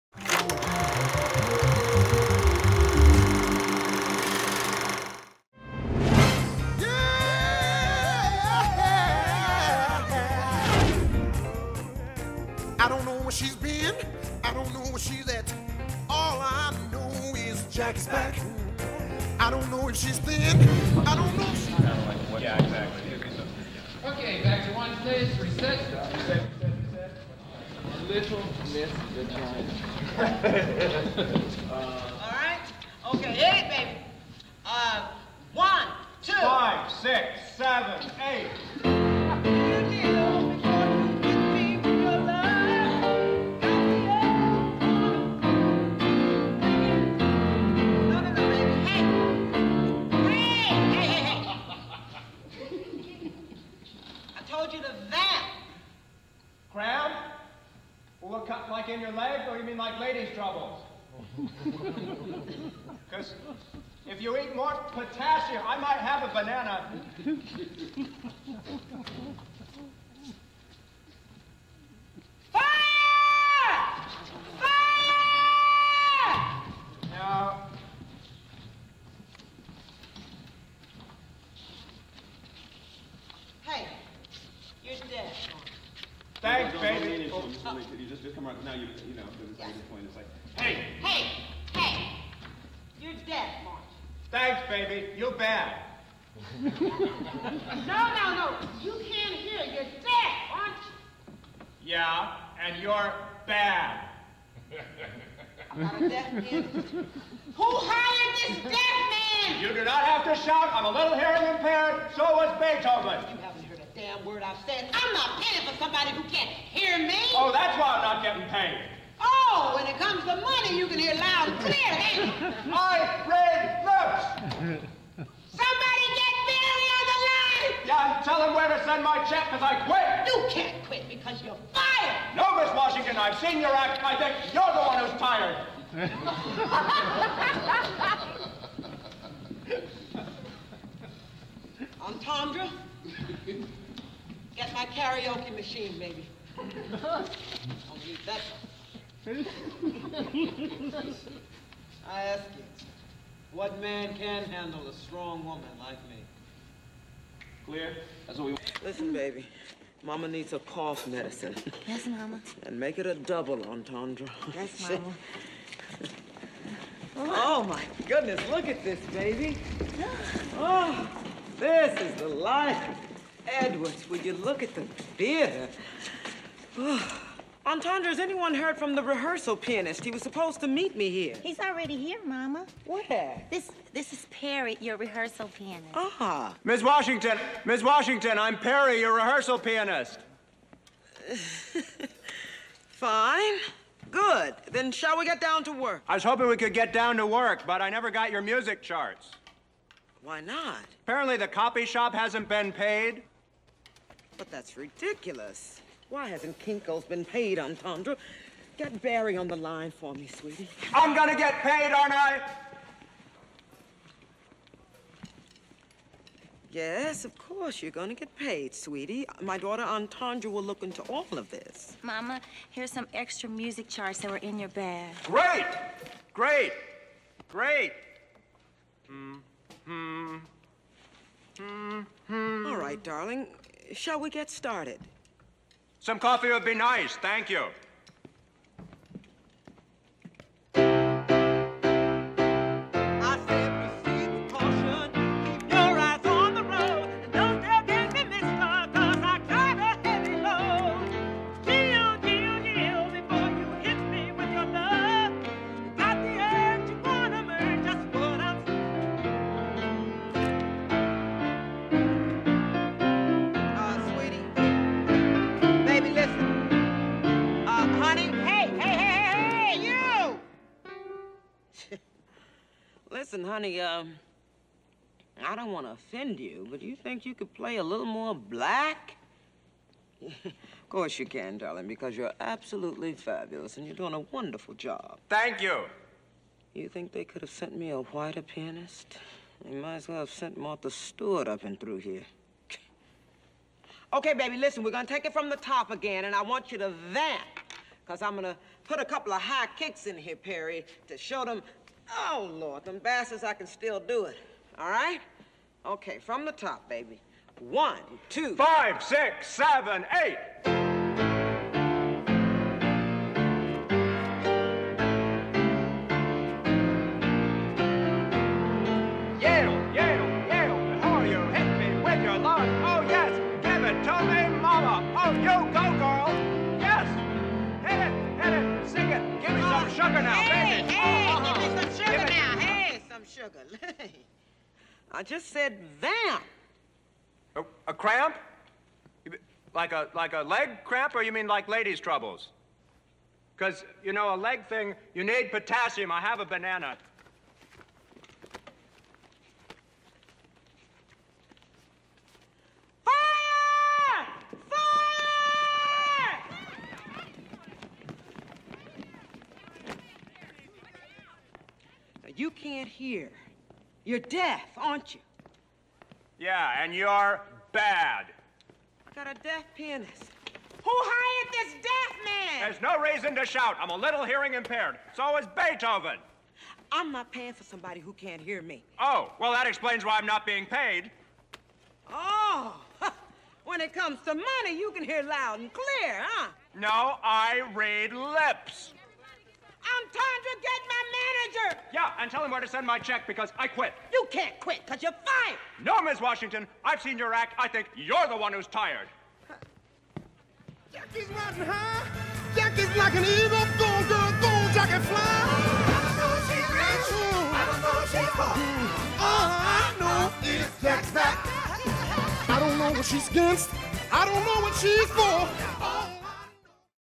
'On Location' Jackie's Back! David Hyde Pierce, Jenifer Lewis, Tim Curry Wilshire Ebell Theatre
#21 in my "On Location" series, a collection of videos created from analog footage shot with a camcorder on TV & Movie production sets I worked on as a script supervisor in the 1990's and 2000's. This is another from the production of the TV "Mockumentary" Jackie's Back (1999)...this one is from one of our many days at the historic Wilshire Ebell Theatre in Los Angeles and includes our 2 main leads, Jenifer Lewis & Tim Curry and also the scenes we shot with David Hyde Pierce as a deaf rehearsal pianist. Director Robert Townsend is also seen during our 1st "blocking" rehearsal